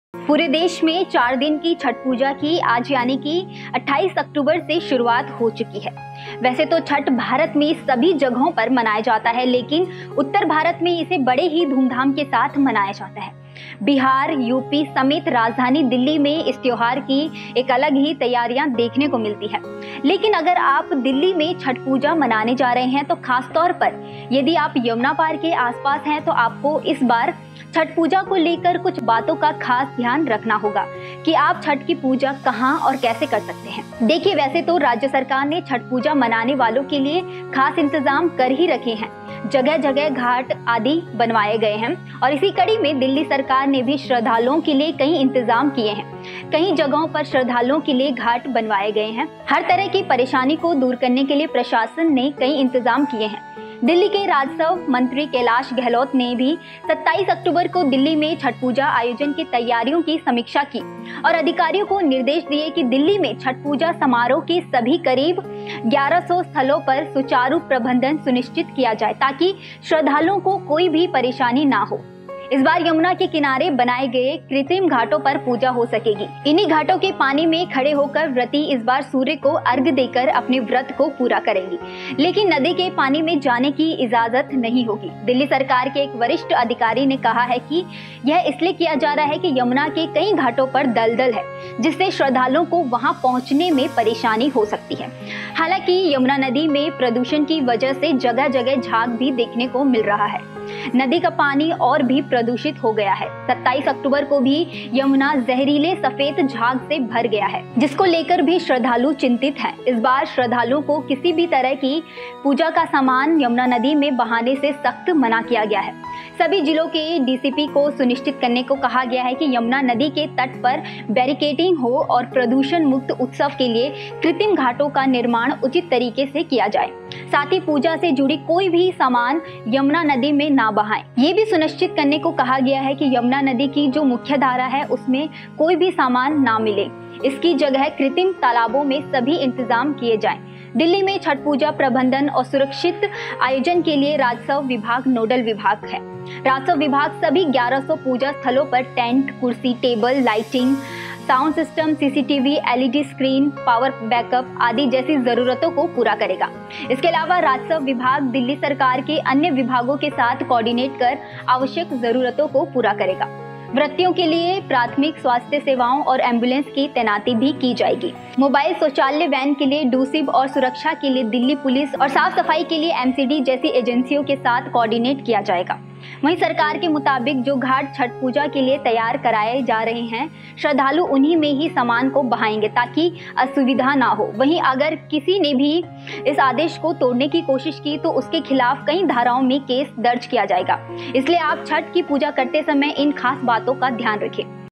यमुना घाट पर नहीं होगी छठ पूजा दिल्ली सरकार का बड़ा फैसला | Chhath Puja 2022
Chhath Puja 2022: दिल्ली सरकार का बड़ा फैसला, यमुना घाट पर नहीं होगी छठ पूजा।

पूरे देश में 4 दिन की छठ पूजा की आज यानी की 28 अक्टूबर से शुरुआत हो चुकी है। वैसे तो छठ भारत में कई जगहों पर मनाया जाता है ।लेकिन उतर भारत में इसे बड़े ही धूमधाम के साथ मनाया जाता है। बिहार, यूपी समेत राजधानी दिल्ली में इस त्योंहार की एक अलग ही तैयारियां देखने को मिलती है।

TODAY Live digital  NEWS with KNOWLEDGE